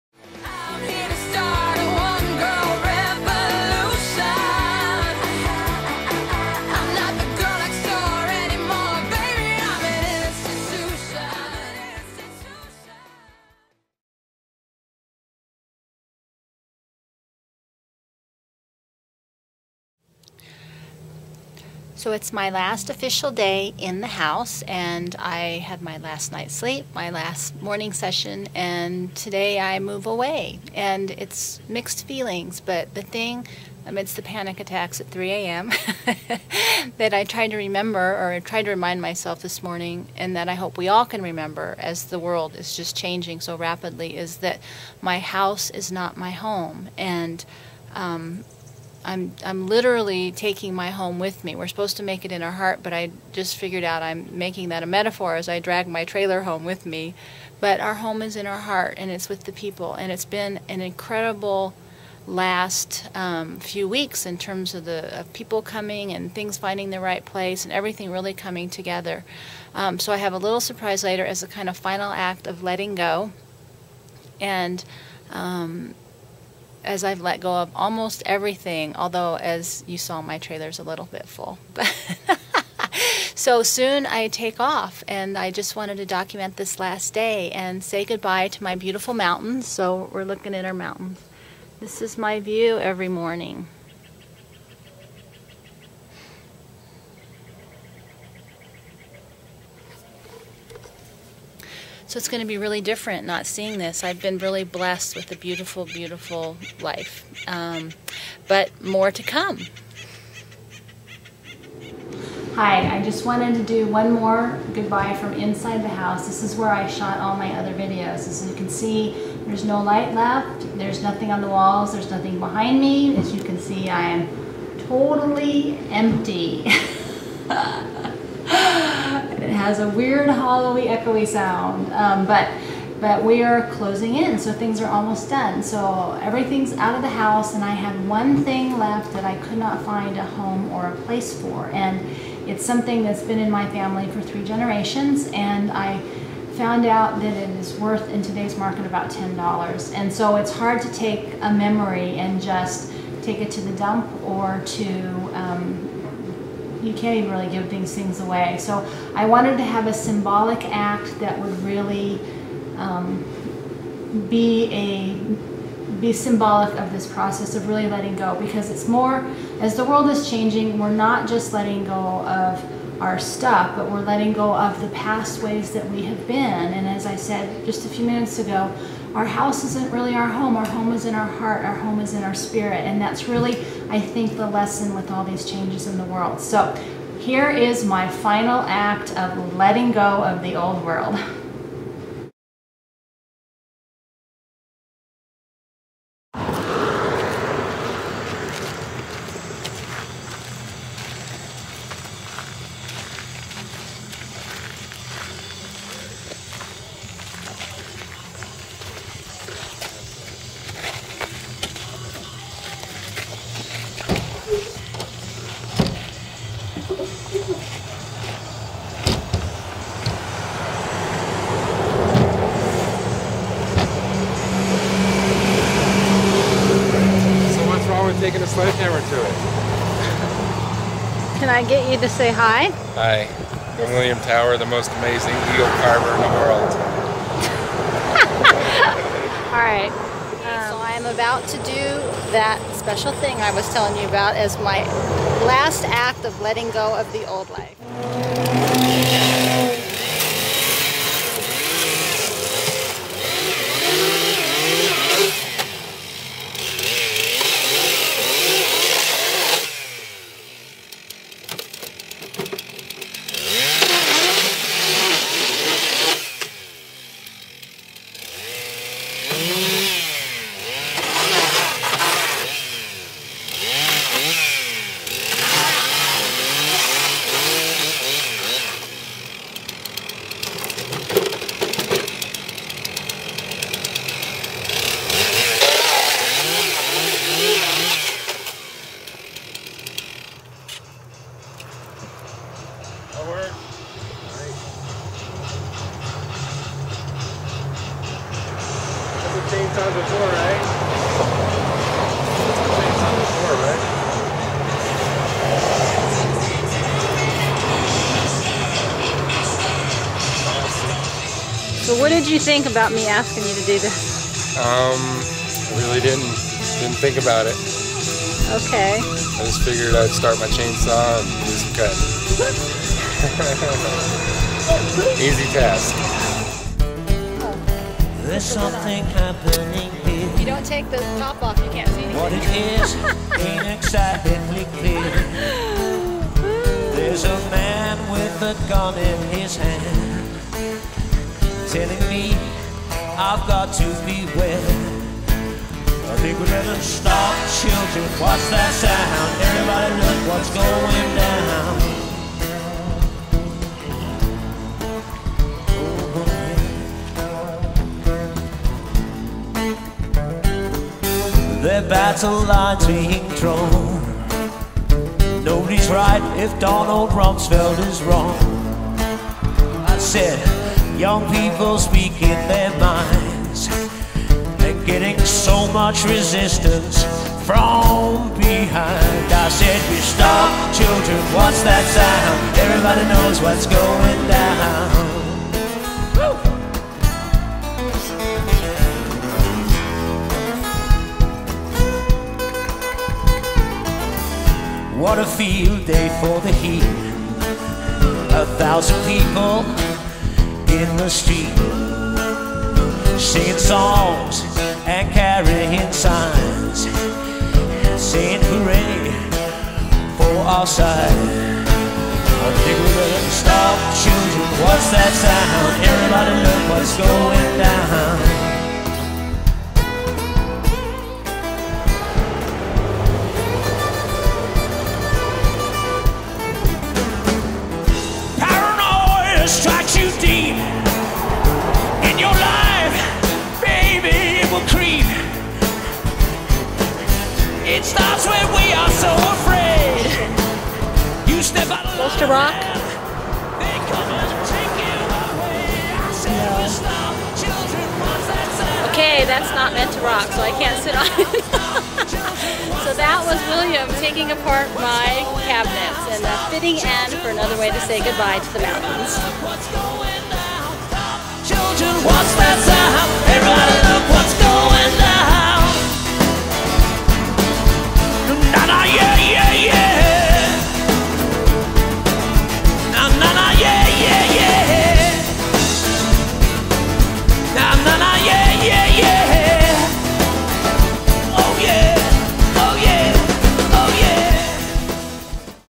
JJ #10 - Letting Go of the Old World
Last day in the old house, transitioning to the new life without one!